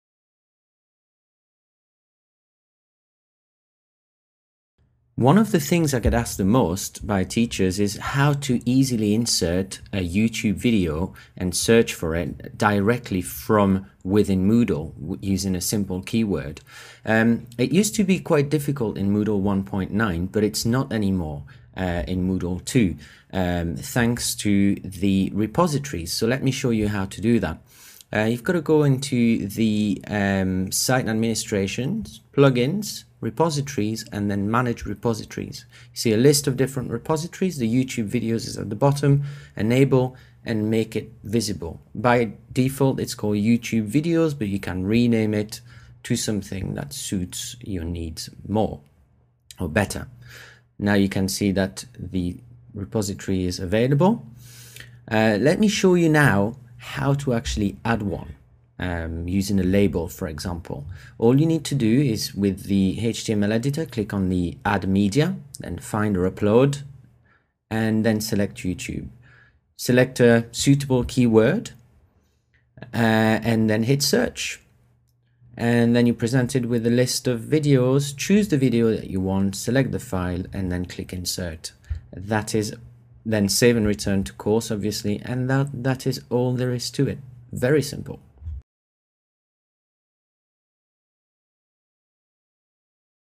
Moodle 2.0 Admin - How to add YouTube videos in Moodle
It is now very easy for teachers to add YouTube videos to their Moodle courses. This video shows admins how to enable this feature in Moodle 2 using the file repositories. It also shows teachers how to add a YouTube video in their courses/resources. This video is best watched at full 720p resolution.